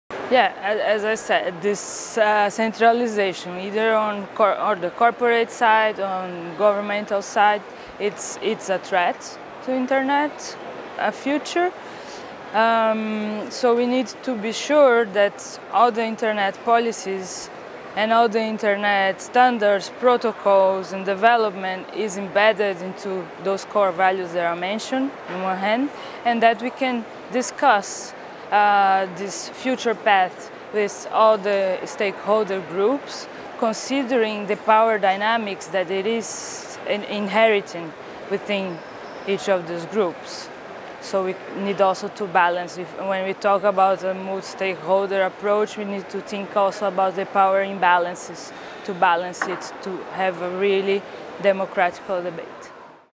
Global IGF 2015: Joana Varon on Biggest Net Threat
Joana Varon, founder and director of Creative Chaos Catalyst, shares her answer to the question, “Experts say one of the biggest threats to the Internet is the ways in which businesses and governments seek to control people’s interactions online. What do you see as the greatest threat to achieving the most positive future for the global Internet?” Recorded at the Global Internet Governance Forum 2015 in João Pessoa, Brazil, by the Imagining the Internet Center, an initiative of Elon University, North Carolina, USA.